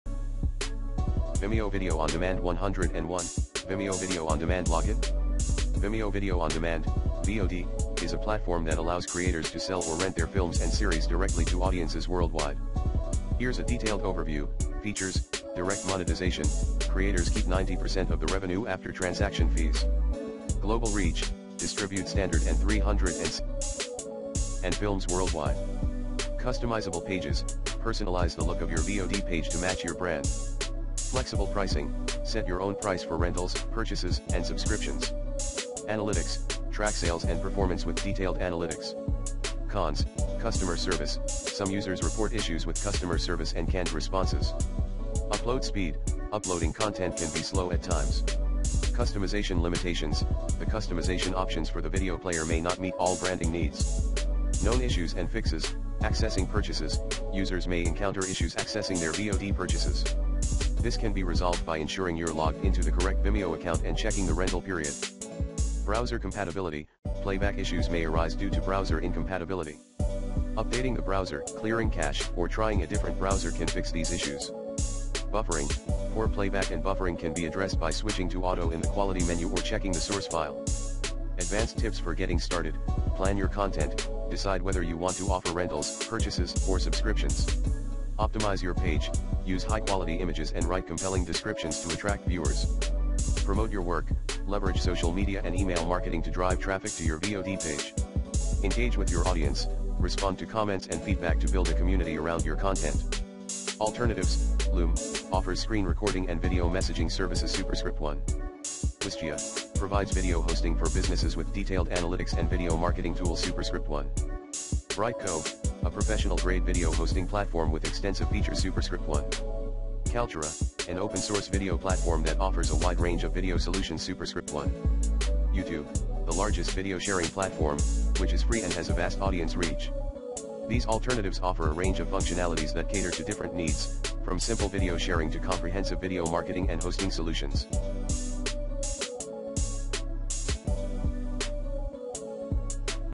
Vimeo video on demand 101: Vimeo video on demand login?
This vid helps get started w/ Vimeo video on demand.

i. Vimeo's Video on Demand (VOD) service allows creators to sell or rent their videos directly to viewers. If you are trying to log in to your Vimeo Video on Demand account, here are the steps you can follow:

2. **Access the Login Page**:
   - Click on the "Log in" button usually found in the upper right corner of the homepage.

3. **Enter Your Credentials**:
   - If you signed up with Facebook, Google, or Apple, you can use the respective button to log in.

4. **Navigate to Your Purchases**:
   - Once logged in, hover over your profile picture in the upper right corner and select "Purchases" from the dropdown menu to access your purchased or rented videos.

4. **Navigate to Vimeo On Demand**:
   - After logging in, click on "Manage videos" in the upper right menu, then select "On Demand" to access your VOD settings and analytics.

### Troubleshooting Tips:

- **Forgot Password**: If you cannot remember your password, click on the "Forgot password?" link on the login page to reset it.
- **Account Issues**: If you have trouble logging in or accessing your account, contact Vimeo support for assistance.
Check your email for any verification links from Vimeo.

### Additional Resources:

- **Help Center**: Vimeo has a comprehensive [Help Center] where you can find more detailed guides and troubleshooting tips.
- **Support**: If you need further assistance, you can contact Vimeo support directly through their website.

By following these steps, you should be able to log in to your Vimeo Video on Demand account and access your purchased content or manage your VOD offerings.